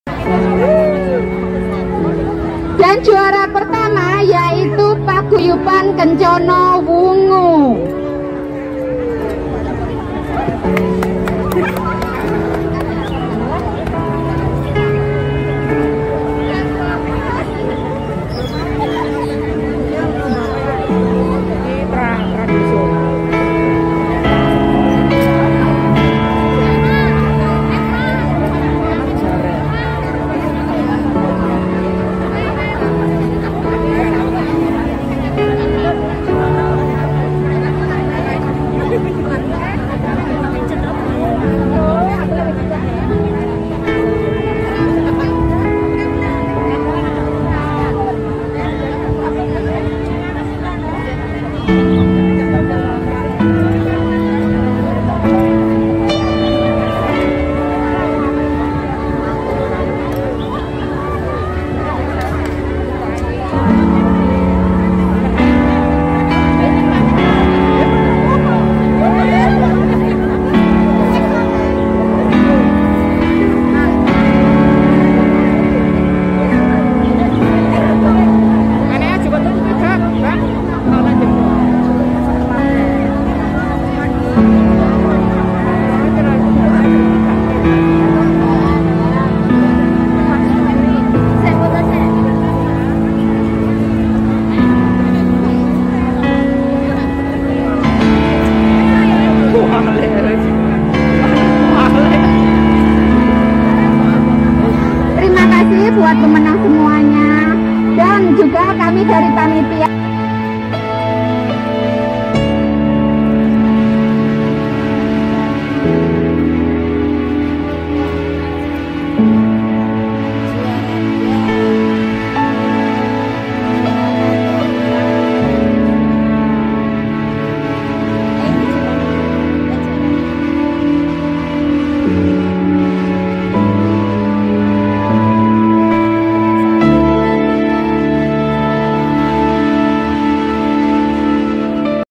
Penyerahan Hadiah oleh PJ KK Anita WK dan Kk Vero Lomba Jaranan dan Tari Tradisional di JBMI
Kegiatan Positif Buruh Migran di Rantauan Hongkong